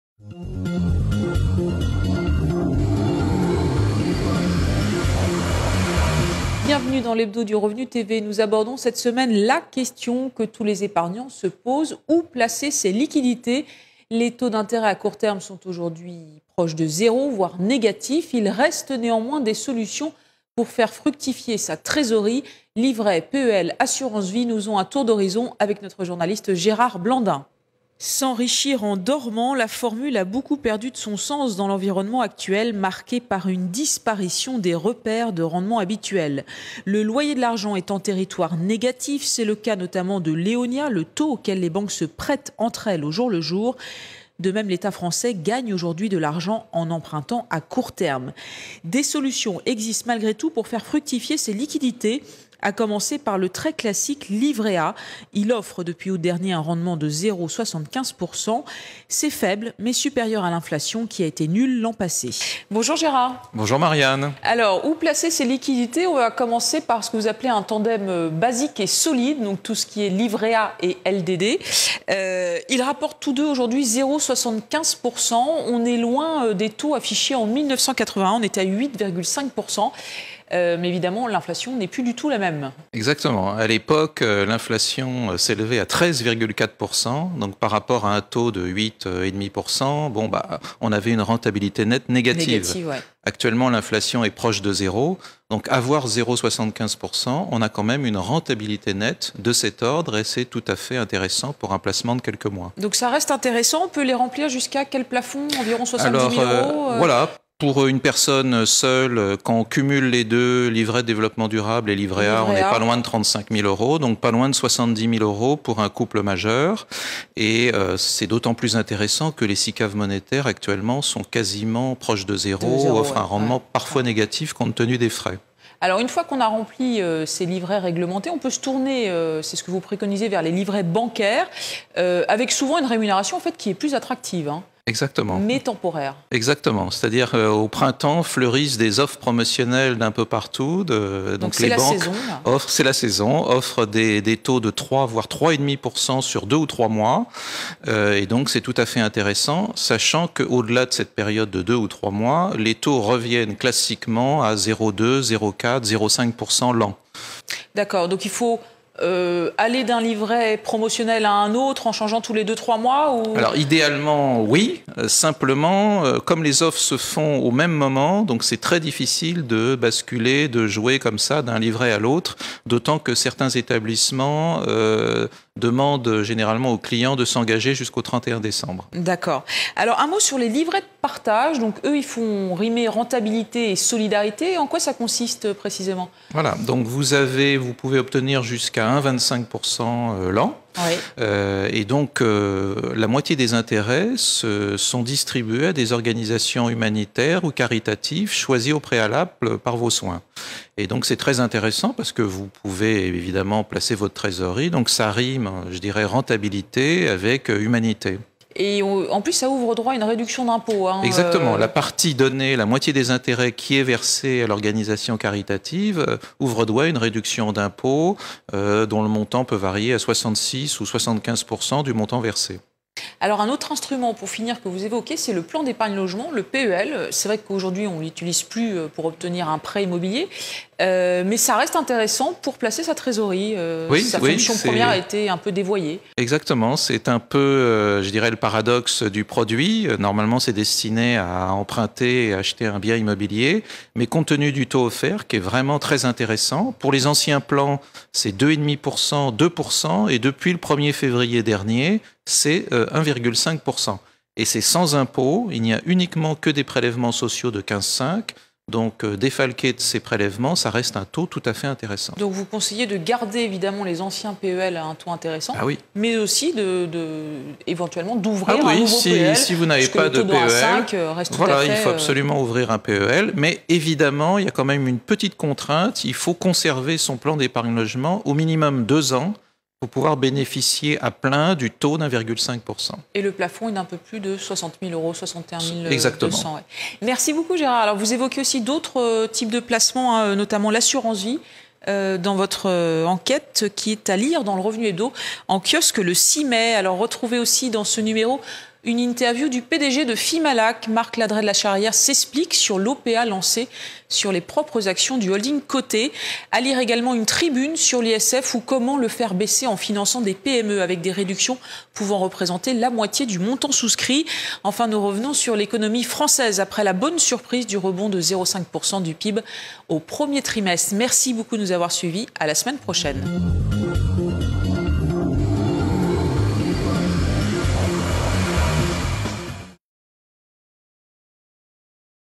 A la Une cette semaine : où placer ses liquidités ?
Retrouvez ici toutes les vidéos produites par la rédaction de lerevenu.com
Avec des taux d’intérêt à court terme aujourd’hui proches de zéro, voire négatifs, faire fructifier sa trésorerie vire au casse-tête pour les épargnants. Il reste néanmoins des solutions, entre livrets (réglementés, bancaires...), assurance-vie ou encore PEL. Tour d'horizon avec Marianne PY et Gérard Blandin